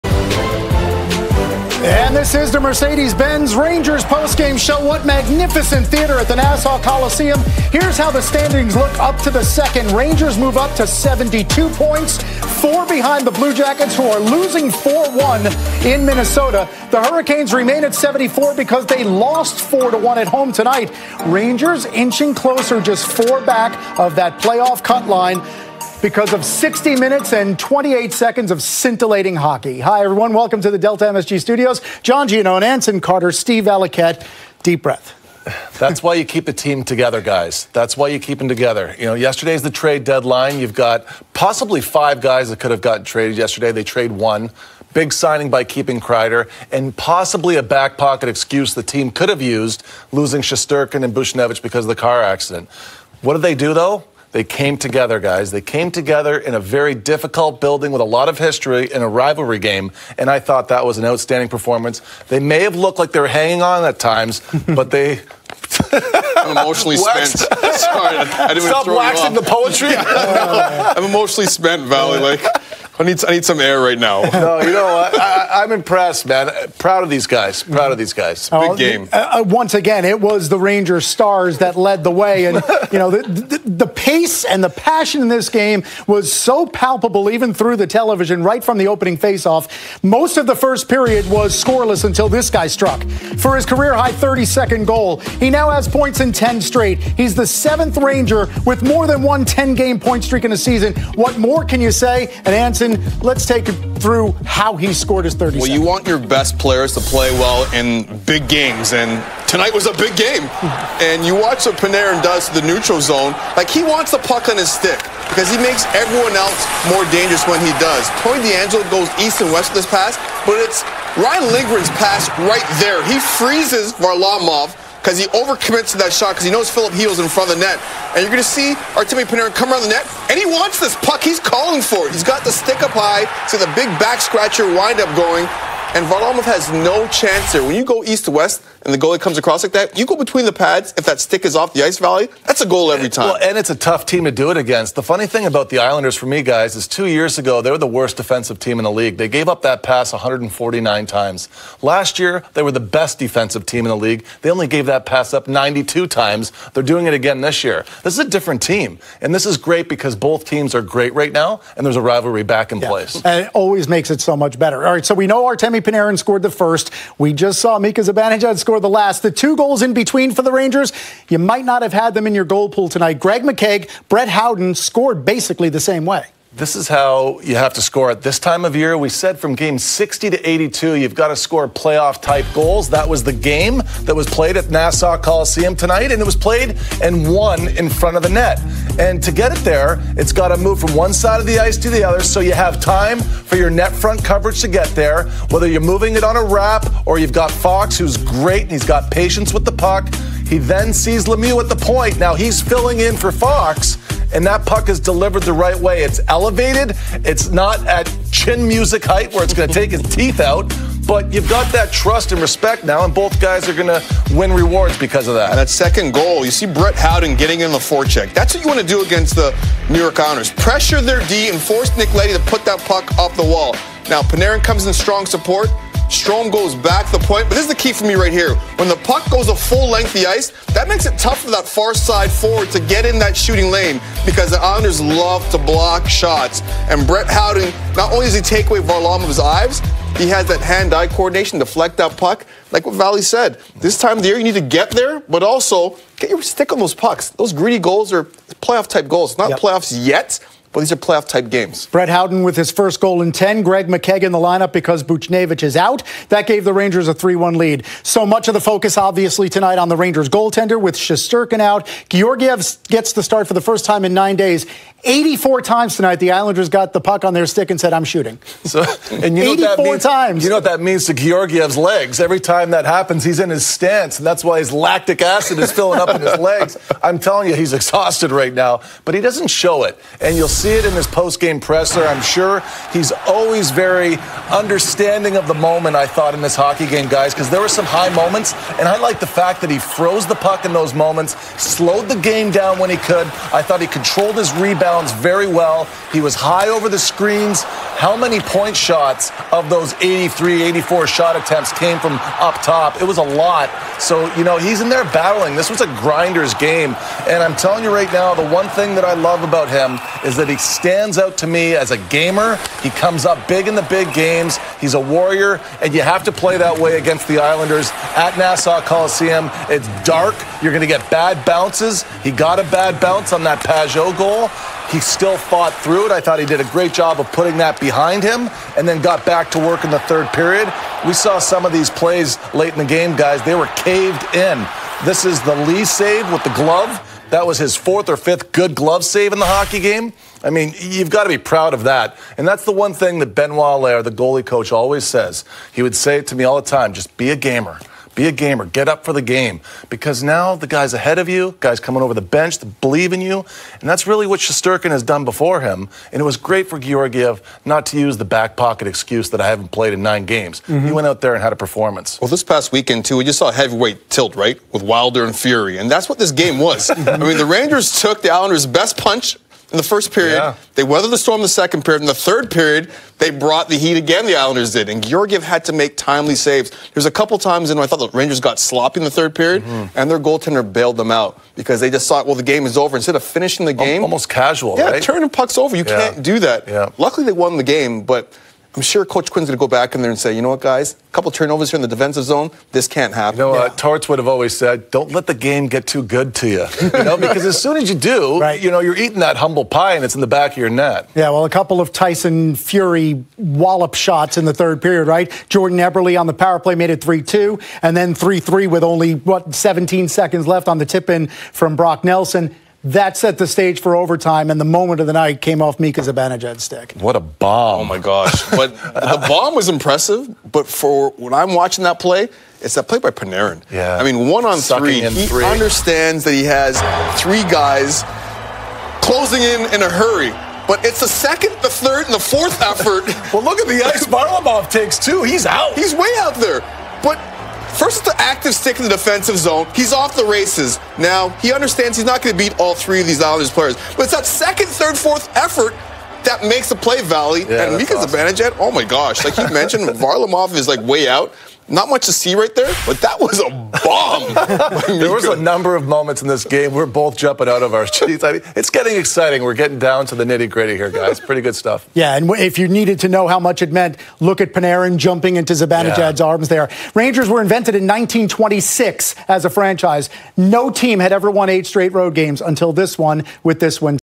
Panarin and Zibanejad Secure Incredible OT Win For Rangers vs. Islanders | New York Rangers
Mika Zibanejad and Artemi Panarin prove they're the Rangers' dynamic duo with a show-stopping overtime game winning goal.